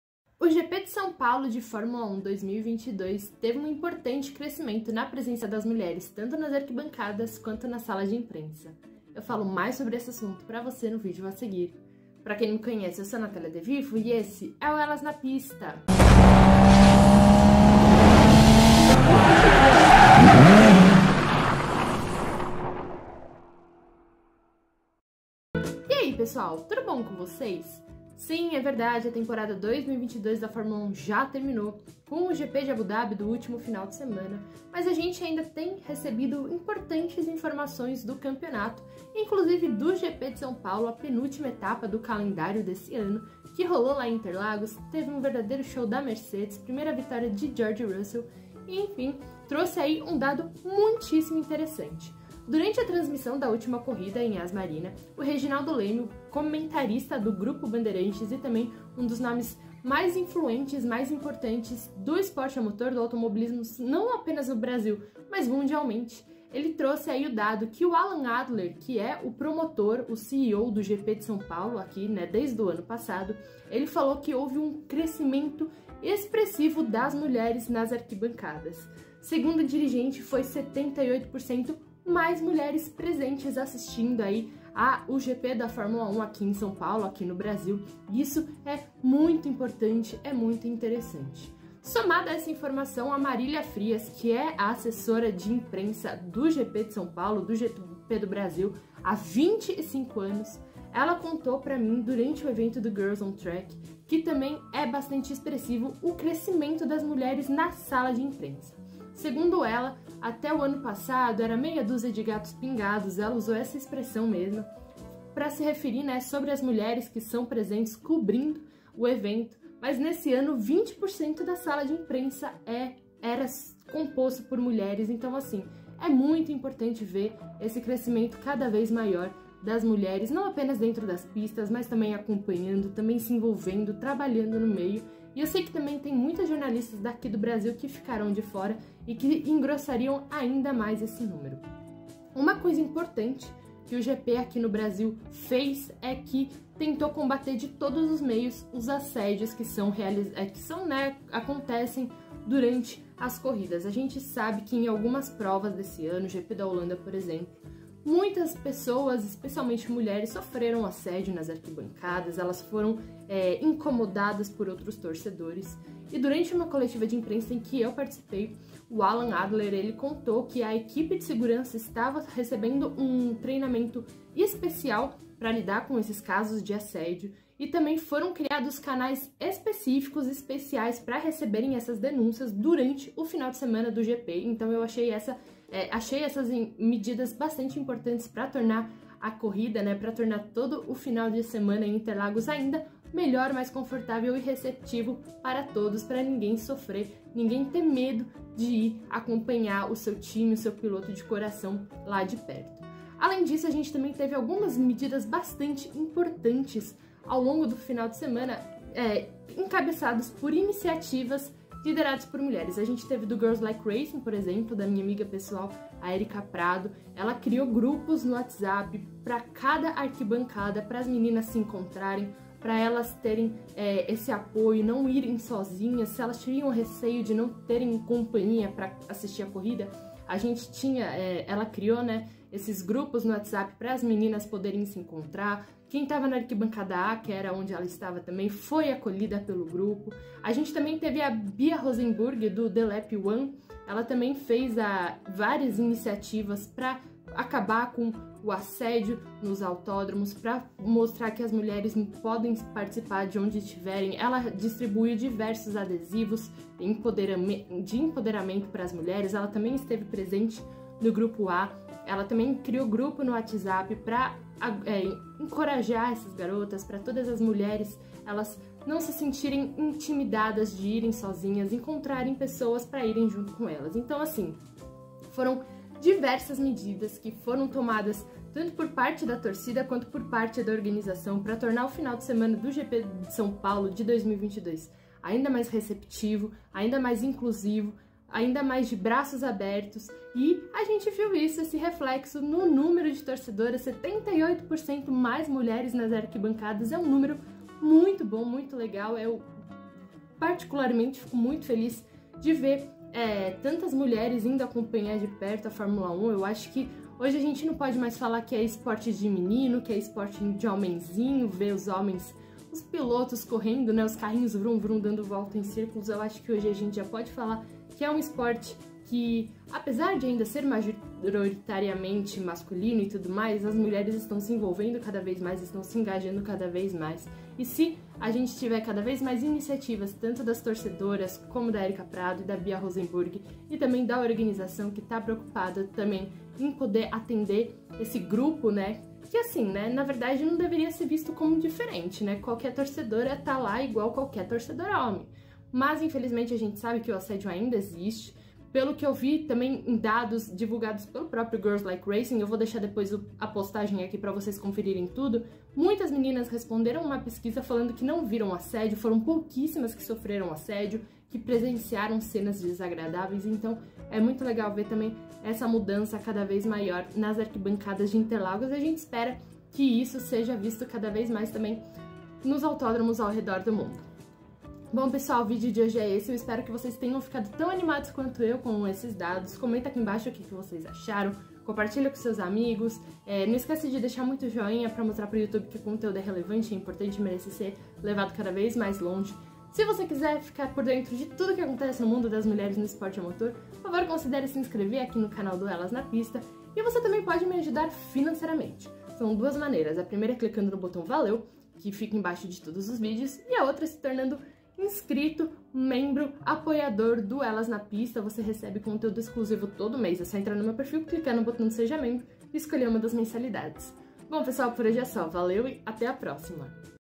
GP DE SÃO PAULO DA F1 TEM GRANDE AUMENTO DE MULHERES
Muito o que se comemorar do GP de São Paulo de 2022 da F1. Segundo a organização, houve grande aumento na presença de mulheres em Interlagos - e puderam se sentir mais seguras com as diversas iniciativas. Saiba mais!

Não se esqueça de curtir, comentar, compartilhar e se inscrever